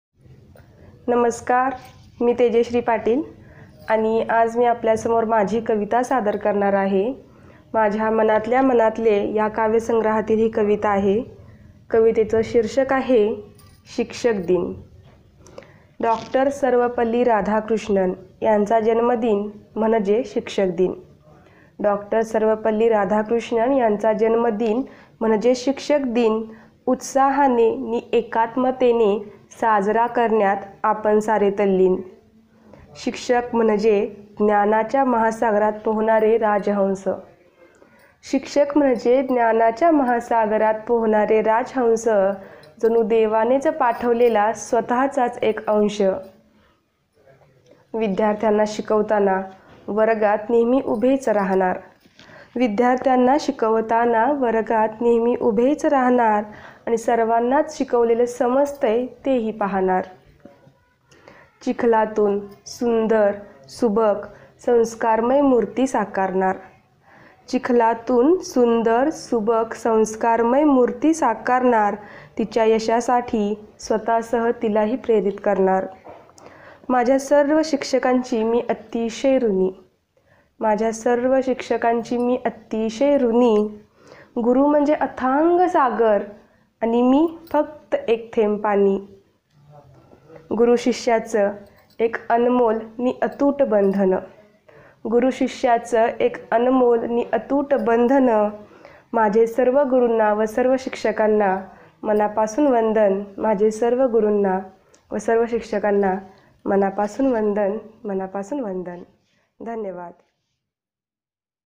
शिक्षक दिन । तेजश्री पाटील । Teacher's Day । Poem by Tejashree Patil । मराठी कविता । Marathi Poem ।
कविता : शिक्षक दिन
कवयित्री : तेजश्री पाटील
काव्यसंग्रह : मनातल्या मनातले

Poem : Teacher's Day
Poet : Tejashree Patil
Poem Book : Manatalya Manatale

मराठी कविता ।
Marathi Poem । Marathi Kavita ।